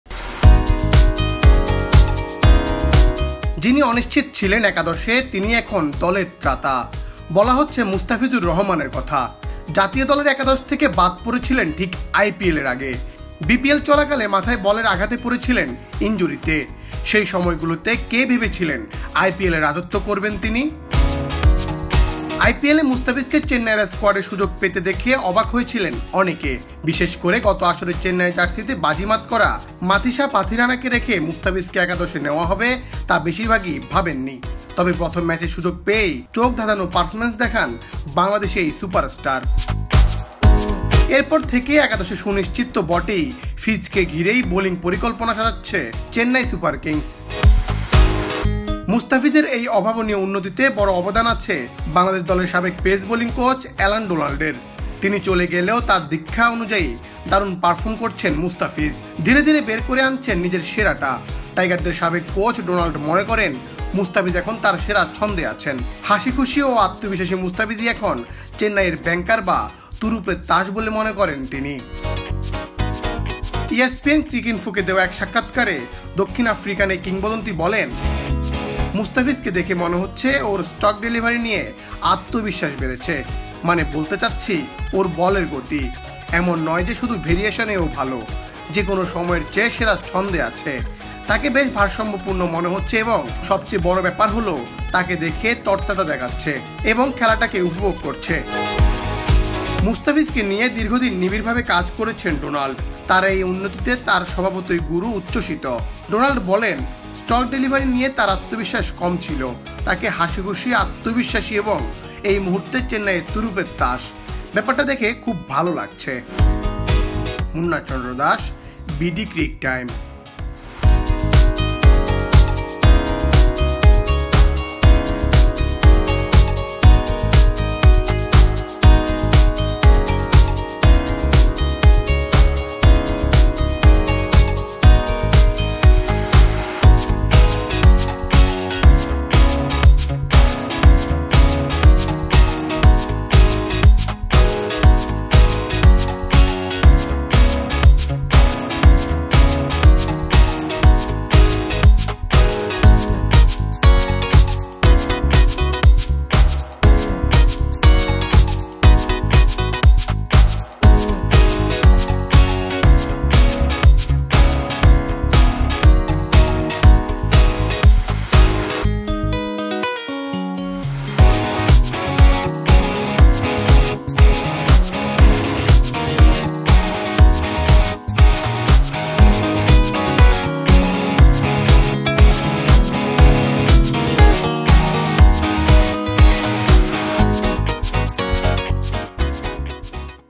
ক্যারিয়ারের সেরা ছন্দে থাকা মুস্তাফিজই চেন্নাইয়ের তুরুপের তাস, মনে করেন ডোনাল্ড | Mustafiz | IPL
যিনি অনিশ্চিত ছিলেন একাদশে, তিনিই এখন দলের ত্রাতা। বলা হচ্ছে মুস্তাফিজুর রহমানের কথা। জাতীয় দলের একাদশ থেকে বাদ পড়েছিলেন ঠিক আইপিএলের আগে। বিপিএল চলাকালে মাথায় বলের আঘাতে পড়েছিলেন ইঞ্জুরিতে। সেই সময়গুলোতে কে ভেবেছিলেন, আইপিএলে রাজত্ব করবেন তিনি? আইপিএলে মুস্তাফিজকে চেন্নাইয়ের স্কোয়াডে সুযোগ পেতে দেখে অবাক হয়েছিলেন অনেকে। বিশেষ করে গত আসরে চেন্নাইয়ের জার্সিতে বাজিমাত করা মাথিশা পাথিরানাকে রেখে মুস্তাফিজকে একাদশে নেওয়া হবে, তা বেশিরভাগই ভাবেননি। তবে প্রথম ম্যাচে সুযোগ পেয়েই চোখ ধাঁধানো পারফরম্যান্স দেখান বাংলাদেশি এই সুপারস্টার। এরপর থেকে একাদশে সুনিশ্চিত তো বটেই, ফিজকে ঘিরেই বোলিং পরিকল্পনা সাজাচ্ছে চেন্নাই।